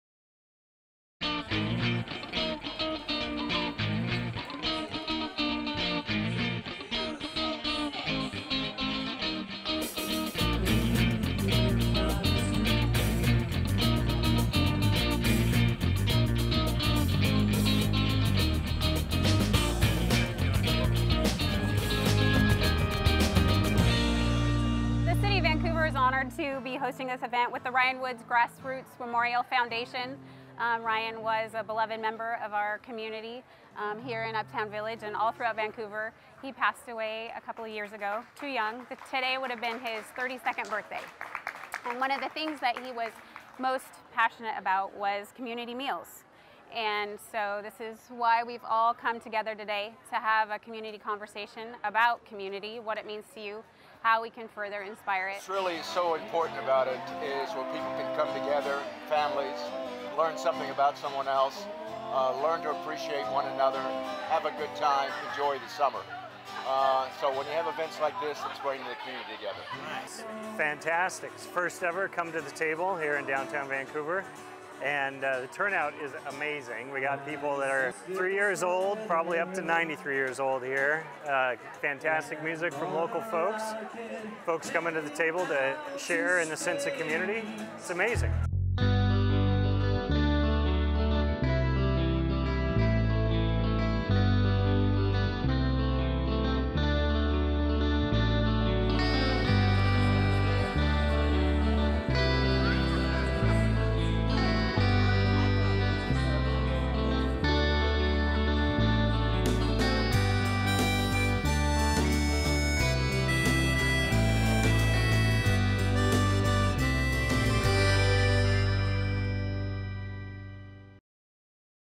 COME TO THE TABLE event in Uptown Village
Watch highlights of the Come to the Table event in Uptown Village. Residents enjoyed a community potluck.